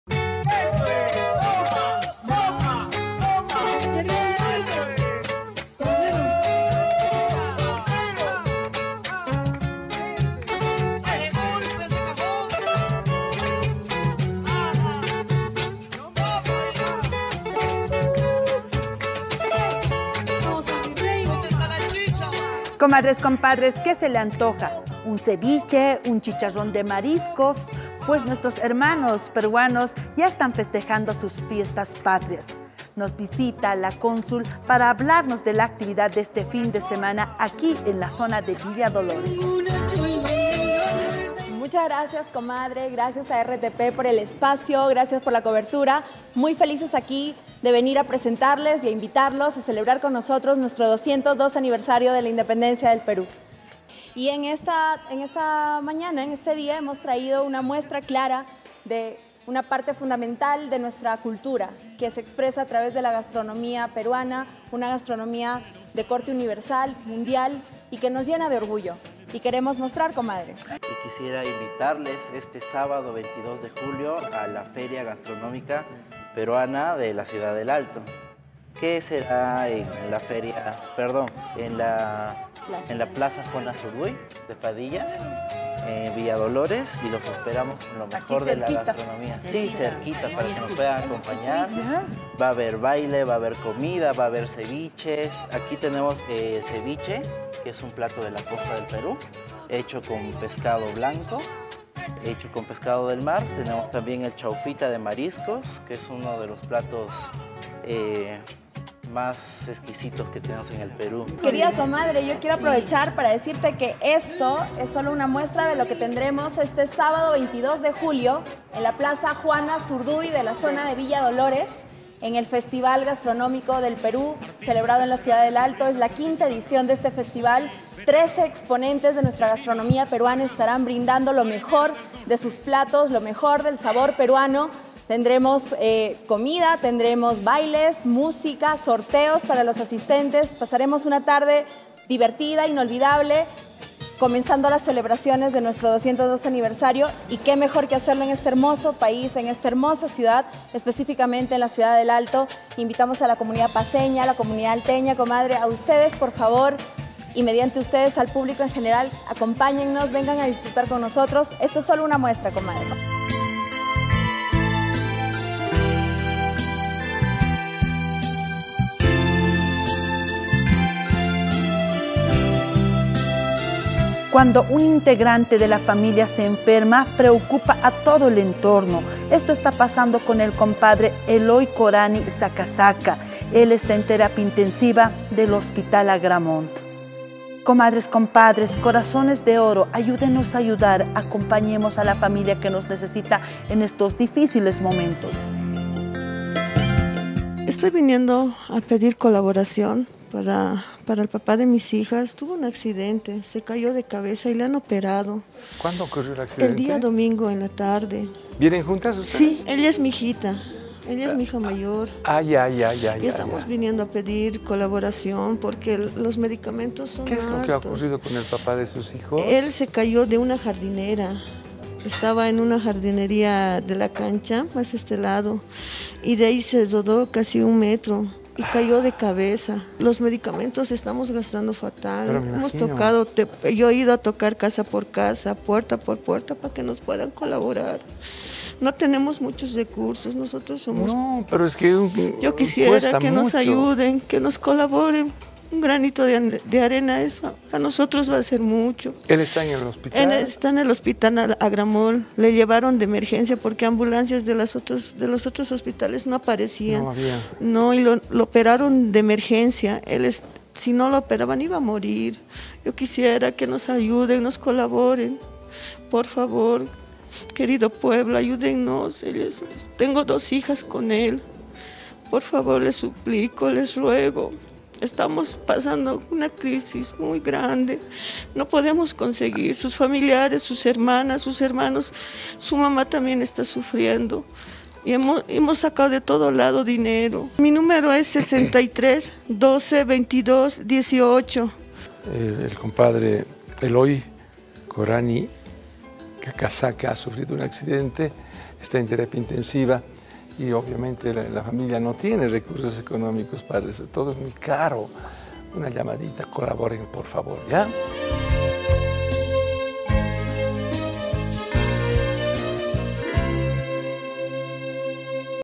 RESUMEN TRIBUNA 20 JULIO
Hoy en La Tribuna Libre del Pueblo nos visito Consulado general del Perú La Paz para invitar al V Festival gastronómico peruano

La comadre Martha Paz pide ayuda para su esposo quién sufrió un accidente y se encuentra en terapia intensiva del hospital Agramont